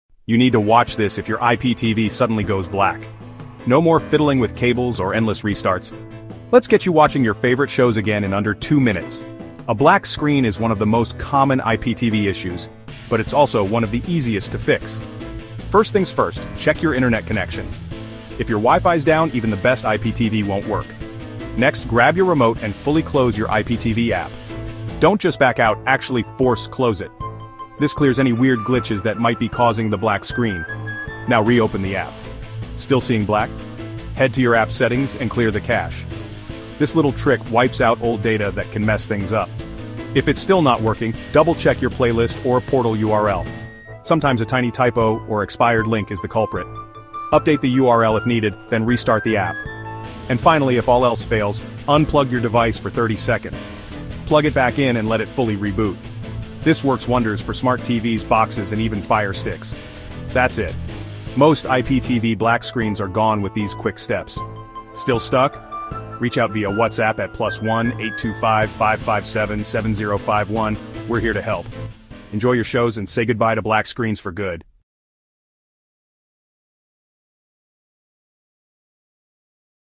IPTV Black Screen Fix – 100% Working Solution!
Having trouble with a black screen on your IPTV?
In this video, we’ll show you the step-by-step solution to fix IPTV black screen issues 100% guaranteed. Whether you’re using IPTV Smarters, XCIPTV, or any other app, this guide will get your channels working again in minutes!

✅ Works on Firestick, Android TV, Smart TV, Windows & Mac
✅ Fix buffering, no signal, and blank screen issues
✅ Tested with VISNOVAA IPTV – Best IPTV Provider 2025